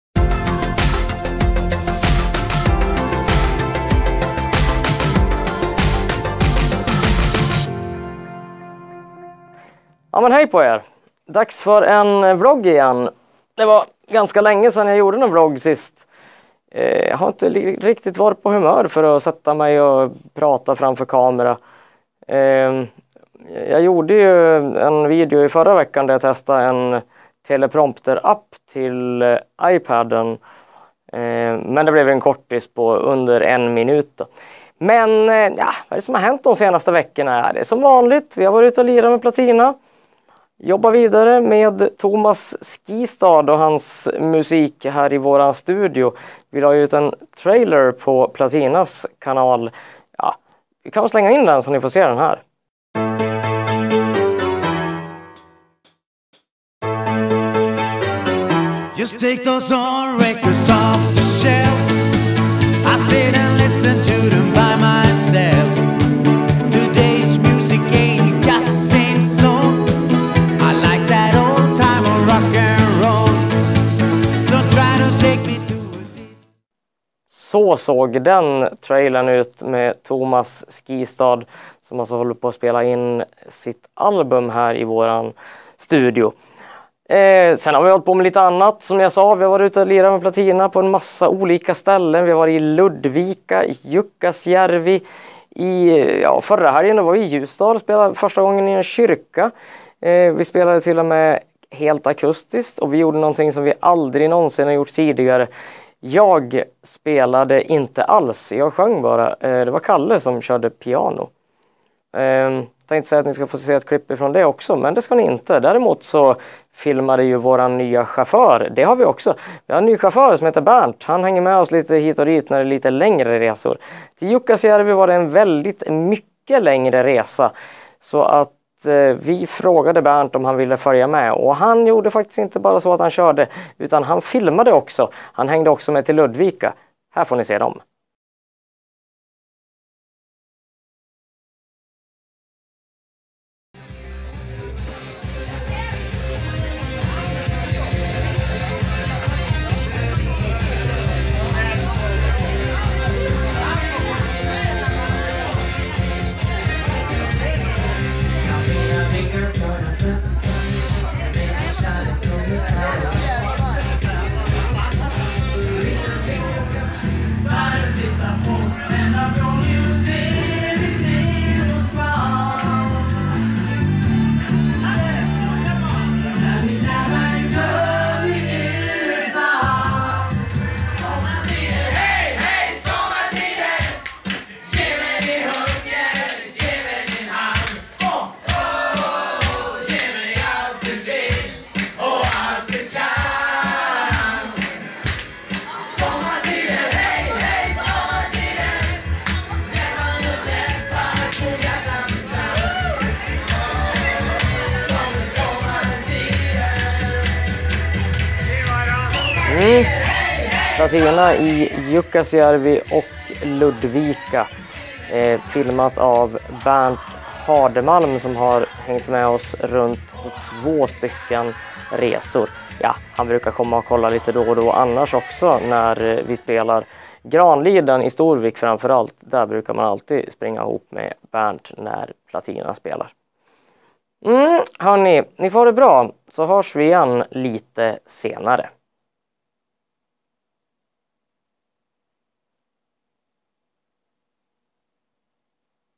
BARA EN UPPDATERING - Johan Bakke Vlogg #54 160906
Det var länge sedan jag gjorde en vlogg sist, så här kommer en uppdatering med vad som har hänt de senaste veckorna!

Jag är annars en musiker som laddar upp blandad musik, både eget och covers.